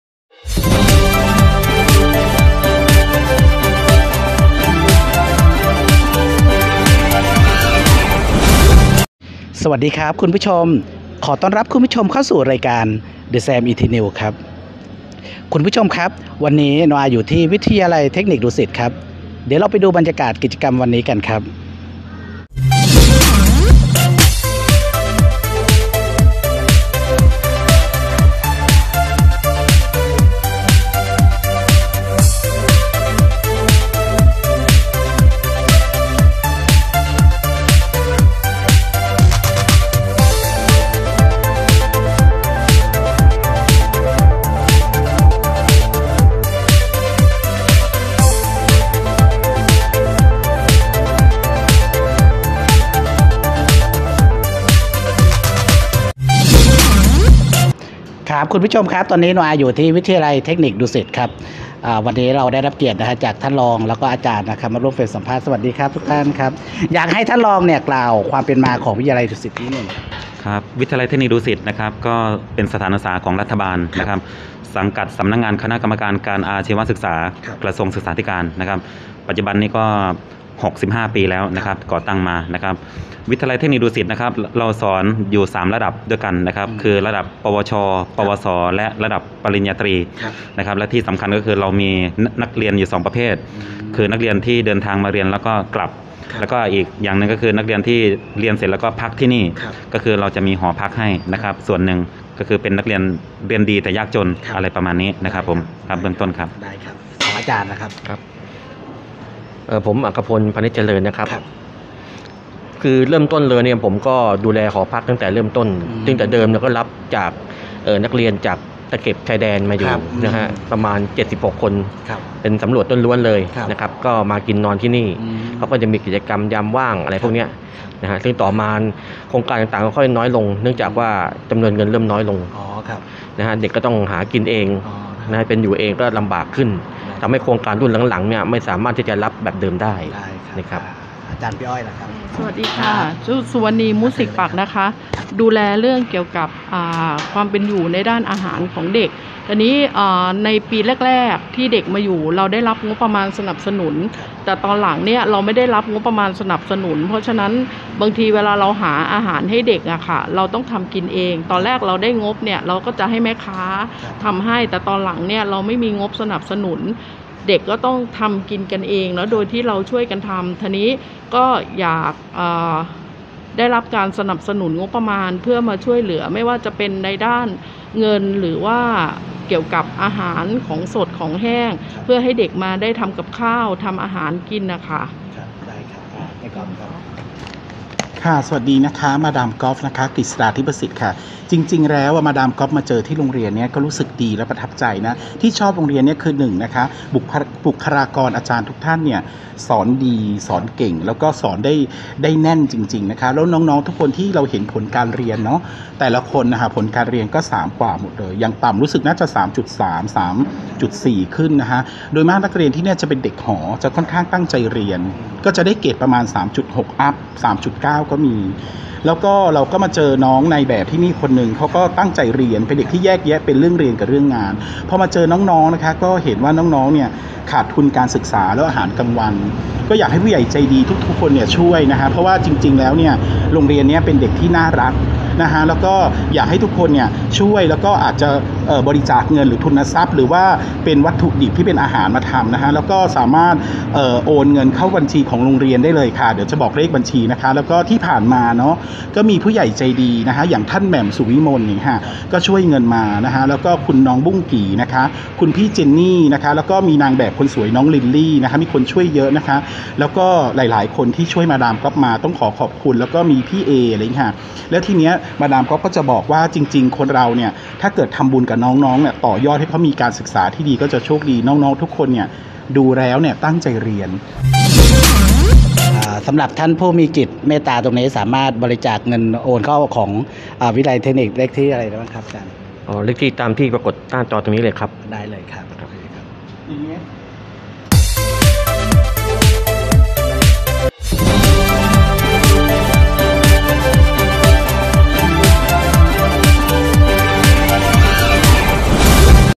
The​ SaM​ ET! NEWS​ : มาดาม​กอล์ฟ​นำทีม​อาจารย์​วิทยาลัย​เทคนิค​ดุสิต​เชิญ​ผู้​มีจิตเมตตา​สมทบทุน
NEWS​ มาดาม​กอล์ฟ​นำทีม​อาจารย์​วิทยาลัย​เทคนิค​ดุสิต​เชิญ​ผู้​มีจิตเมตตา​สมทบทุนช่วยอาหารกลางวัน​แก่นักศึกษา​

4 Ku-band @ 54 MHz
24 Ku-band @ 27 MHz

Downlink Information:
NAT TV 1 and 2
Transponder: 22
Frequency: 12090
Symbol Rate: 20000
FEC: 3/4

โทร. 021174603